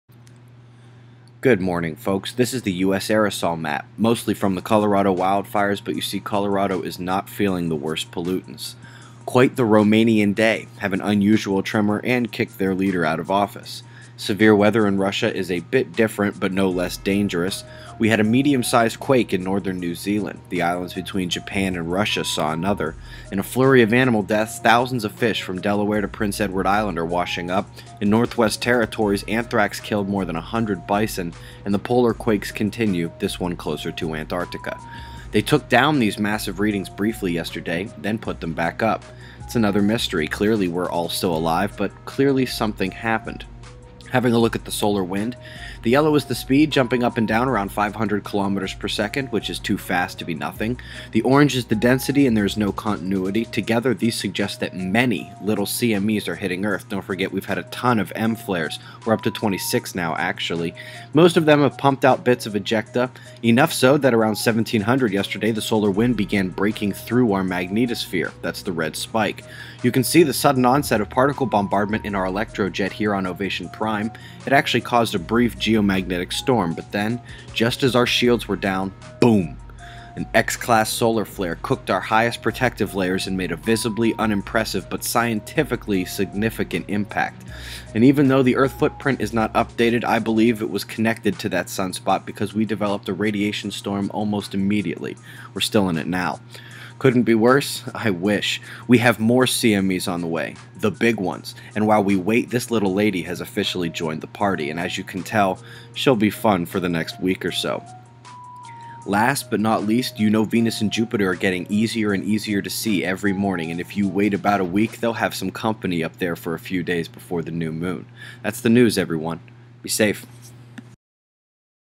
2MIN News July 7, 2012: X-Class Flare, Radiation Storm, Magnetic Storm
China: Xinhua, China.org, Chinadaily
Russia: Ria Novosti
Israel: Jerusalem Post, Israel National News, Haaretz
M/E: ArabNews, AlJazeera